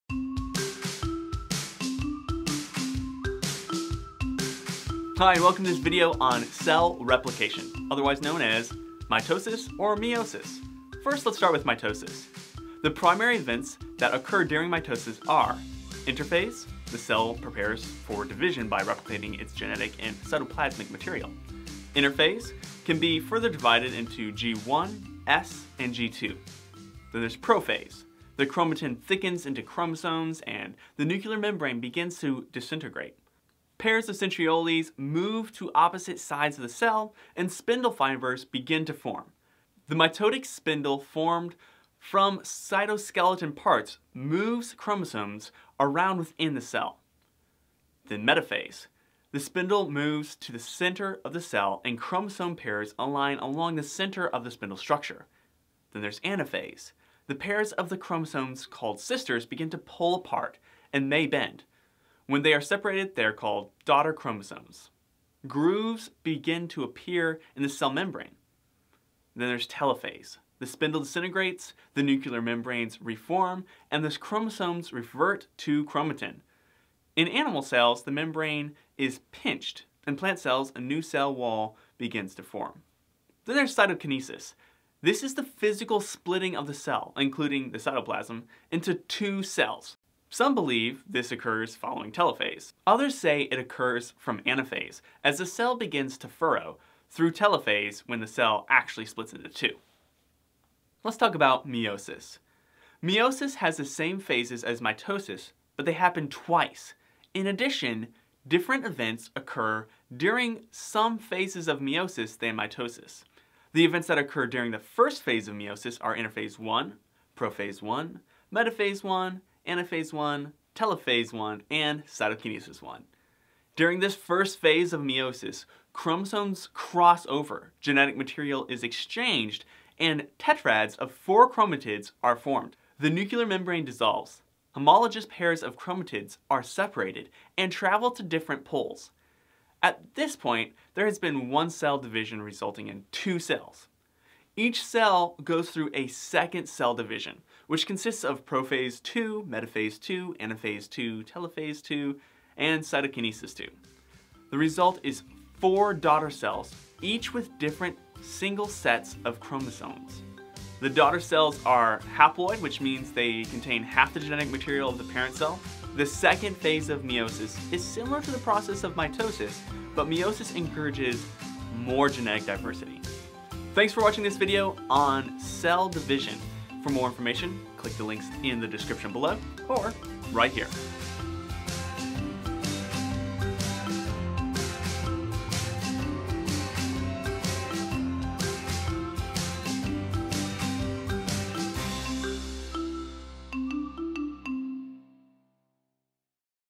Cellular Division
Hi, and welcome to this video on cell replication, otherwise known as mitosis or meiosis.